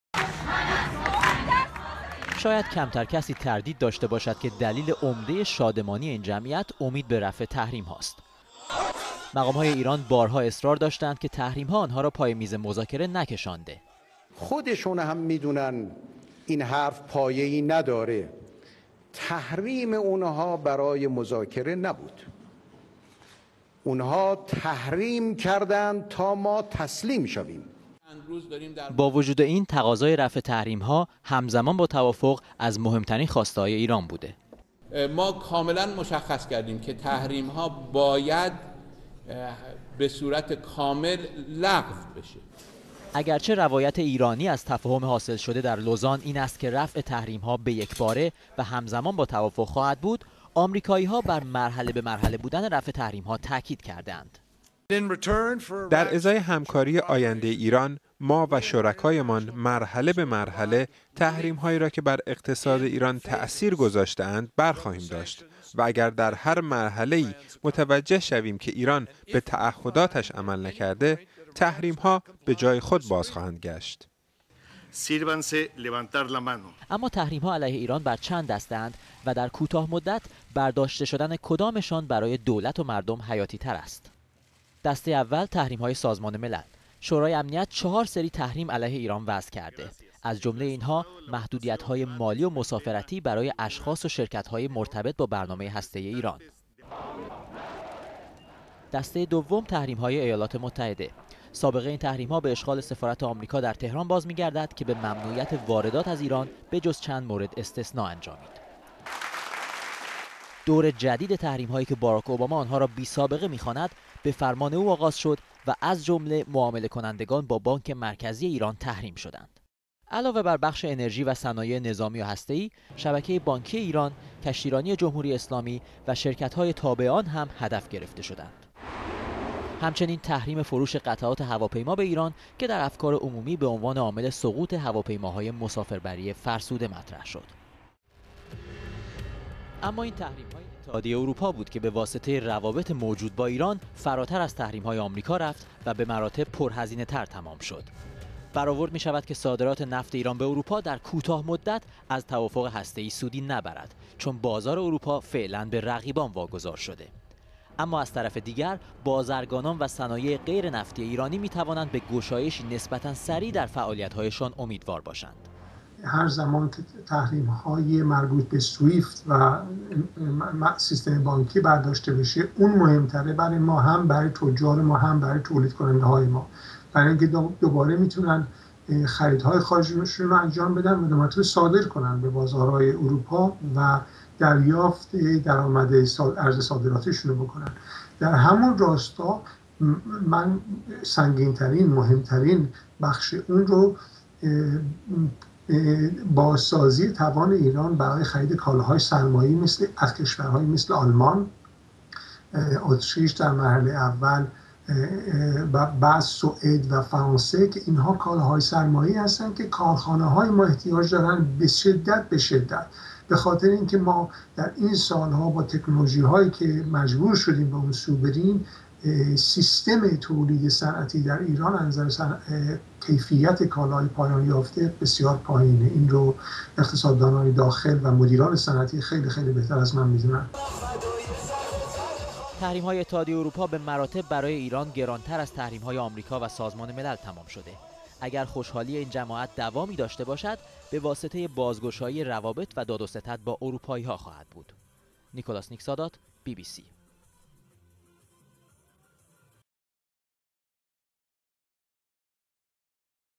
امید به پایان سختی های اقتصادی ایران در پی تفاهم هسته ای
تحریمهای بین المللی مربوط به برنامه هسته ای ایران بعد از اجرای مفاد توافق نهایی لغو یا معلق می شود. اما تحریمهایی که ایران را به دلایل حمایت از تروریسم، نقض حقوق بشر و برنامه موشکی هدف قرار داده همچنان پابرجا می ماند.